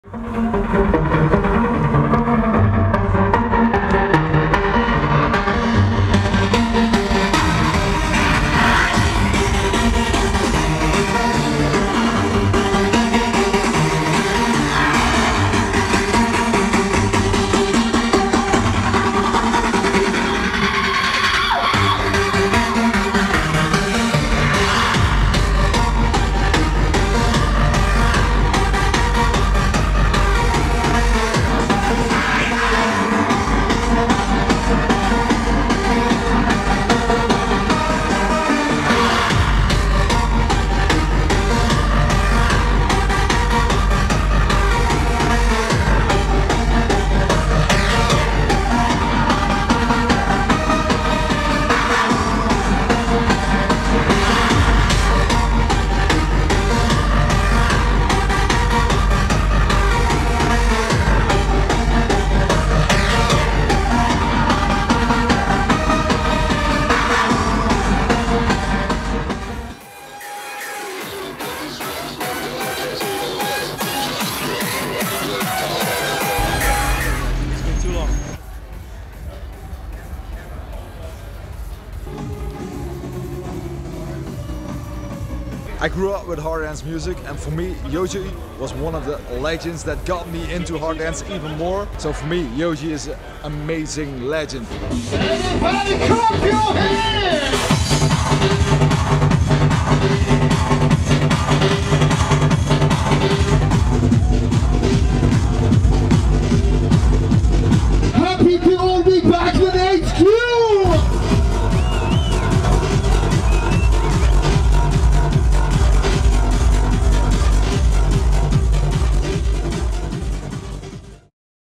YOJI BIOMEHANIKA at Dance Valley 2017 HQ Stage After Movie
Dir/Camera/Edit : Makoto Miyauchi
Assistant Camera : Kim Se Hee
Assistant Edit : Saki Adachi
Special Thanks : Ona Kaneko

You can listen to the set "YOJI BIOMEHANIKA - DanceValley2017 [HQ] RECUP"